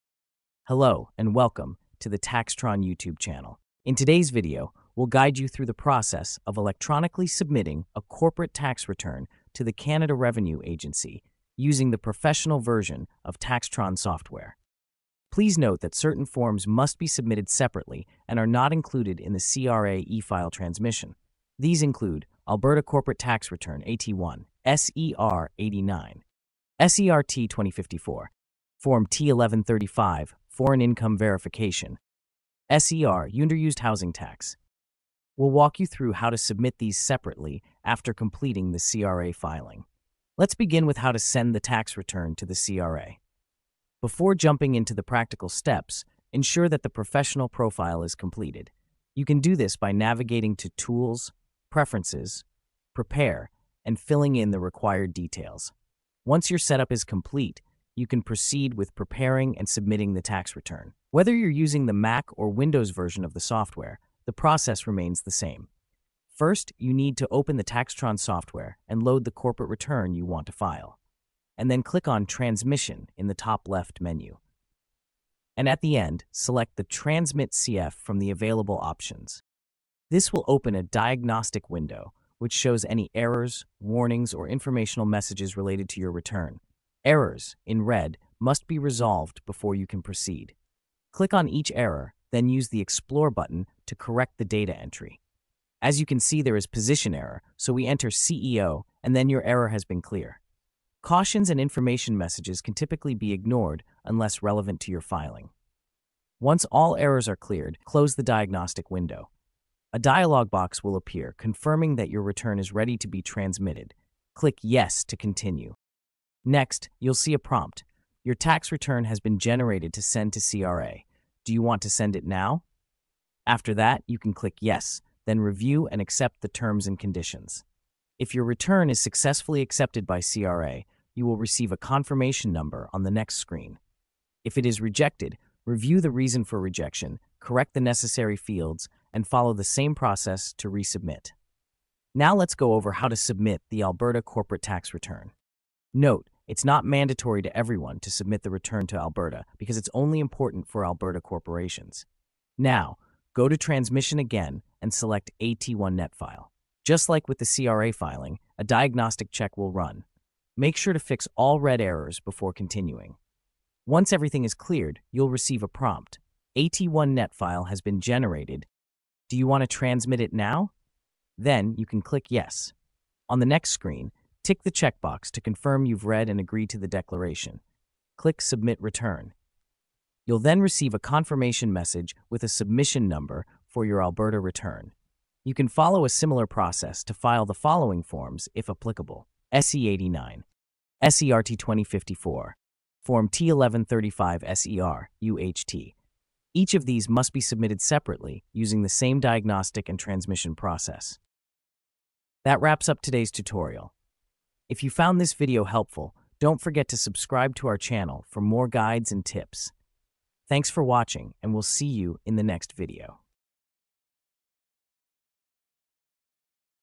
How to E-File a Corporate Tax Return with TaxTron (T2) | CRA & Alberta Filing Guide
Welcome to the official TaxTron YouTube channel!
In this video, we’ll guide you step-by-step through the process of electronically submitting a corporate tax return (T2) to the Canada Revenue Agency (CRA) using the professional version of TaxTron software.

Whether you're using Mac or Windows, this tutorial applies to both platforms and is designed for tax professionals and corporate filers.

🔹 What You'll Learn in This Video:

How to e-file a T2 return with CRA using TaxTron
Key forms that must be submitted separately
How to complete the professional profile setup
How to run and resolve diagnostic checks before transmission
How to submit the Alberta Corporate Tax Return (AT1)
Overview of submitting forms like T1135, SER 89, T2054, and UHT

📌 Important Notes:
Some forms are not included in the CRA e-file transmission and must be filed separately. These include:

Alberta Corporate Tax Return (AT1)
Form T1135 (Foreign Income Verification Statement)
SER 89
SER T2054
SER UHT (Underused Housing Tax)

We cover how to handle each of these in the video.

🌐 Filing Your Return with TaxTron:
✅ Complete your Professional Profile (Tools - Preferences - Prepare)
✅ Use the Transmission - Transmit CIF option for CRA filing
✅ Resolve all diagnostic errors before transmission
✅ Receive a confirmation number after successful CRA or AT1 filing

📩 Need Help?
Explore our channel for more detailed tutorials on TaxTron’s features and tax filing best practices.

💬 Questions or comments? Drop them below—we’re here to help!